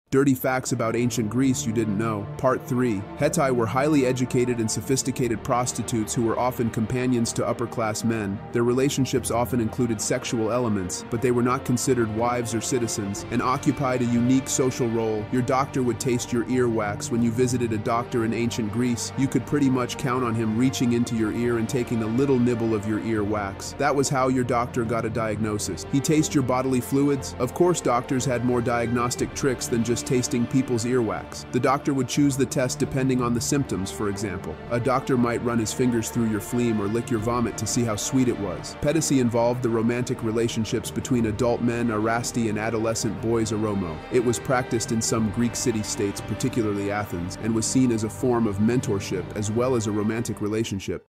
Dirty Facts About Ancient Greece You Didn't Know!
Welcome to a mind-bending journey through the annals of time! In this captivating YouTube short, we dive headfirst into the realm of history, unearthing some of the most mind-boggling and eccentric tales ever recorded. Get ready to have your mind blown as we present a whirlwind of crazy and bizarre historical facts that will leave you astounded!

In this video:
The Roman Emperor Elagabalus as a show of power upon entering Rome to become crowned emperor had hundreds of naked women drag his chariot down the streets of Rome.

'Hysteria' as it was known then – but now is known as anxiety, nervousness or irritability – was considered a serious problem for women during the Victorian period. Doctors, however, discovered that a "pelvic finger massage" could temporarily relieve women of these symptoms by inducing a "hysterical paroxysm."

Ferdinand VII had such an enormous companion that he had to penetrate his wife using a pillow with a hole in it. He had to go through several wives until he found one who could handle him. His doctor wrote in his diary in a very graphic way how big and malformed his companion was."

George II died on the chamber pot after farting so hard he burst a blood vessel.
Julie D’Aubigny was a famous 17th century French opera singer who once took the holy vows to enter a convent just so she could have sex with a nun.